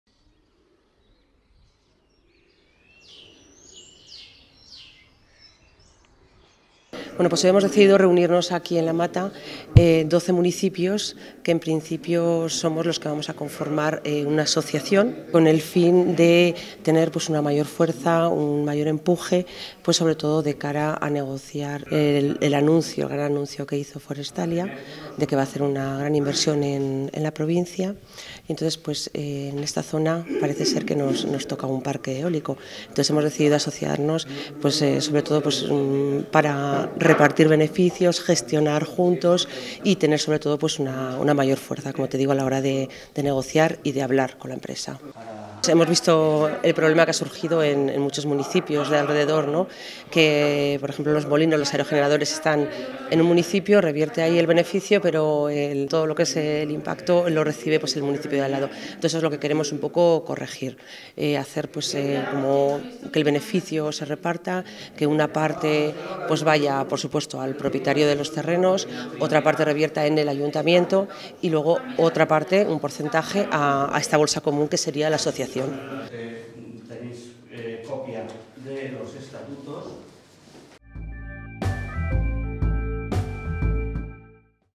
Doce pueblos se asocian para explotar los parques eólicos de Forestalia
Los alcaldes de Aliaga, Ejulve, Cañizar del Olivar, Molinos, Los Olmos, Berge, Palomar de Arroyos, La Zoma, Gargallo, La Mata de Los Olmos, Andorra y Seno se reúnen este jueves en La Mata para constituir la Asociación Majalinos Renovables, creada para rentabilizar de forma conjunta los parques eólicos y fotovoltaicos anunciados recientemente por la empresa Forestalia.